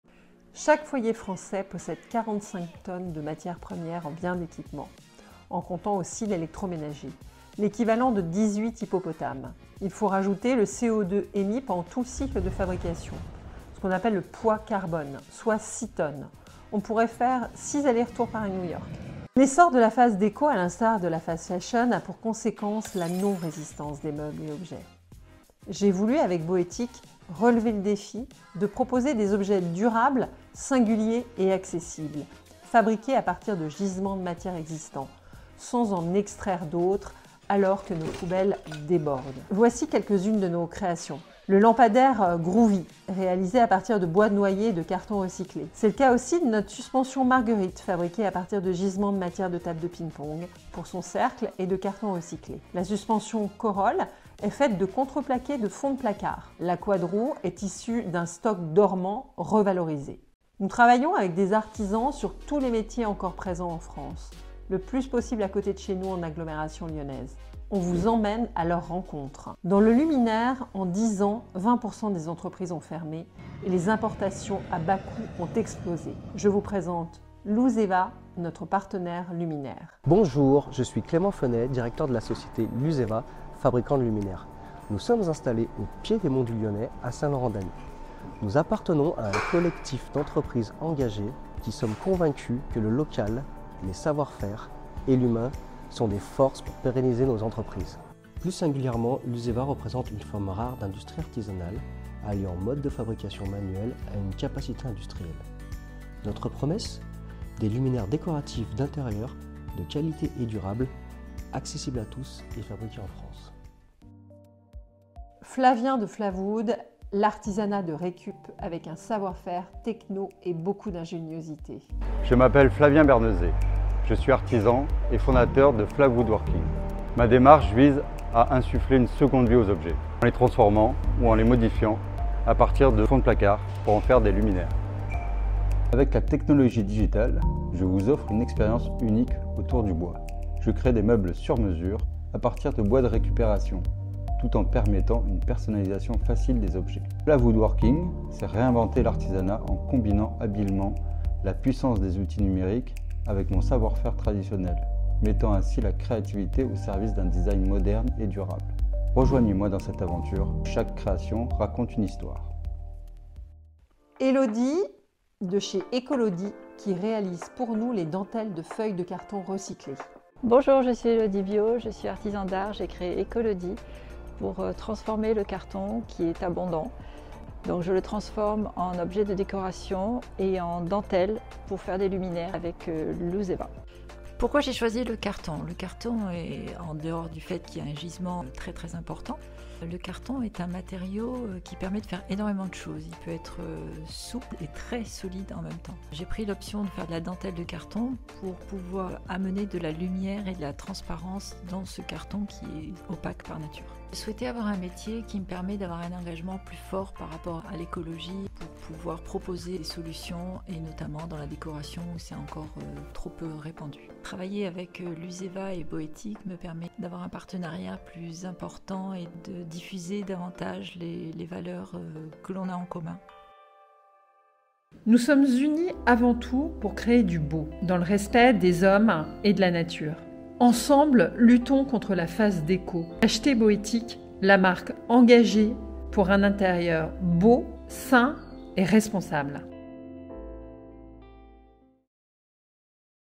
Boethic - Luzeva partenariat luminaires
Présentation de mon nouveau partenariat avec Boethic et Luzeva des engagements pour une décoration éco-responsable.